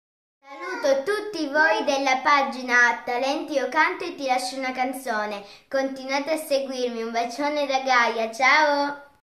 Gaia Saluta tutti i Fans della Pagina "Talenti Io Canto e Ti Lascio Una Canzone"!
Gaia è una bambina che ,attualmente sta partecipando all'Ottava Edizione di "Ti Lascio Una Canzone"!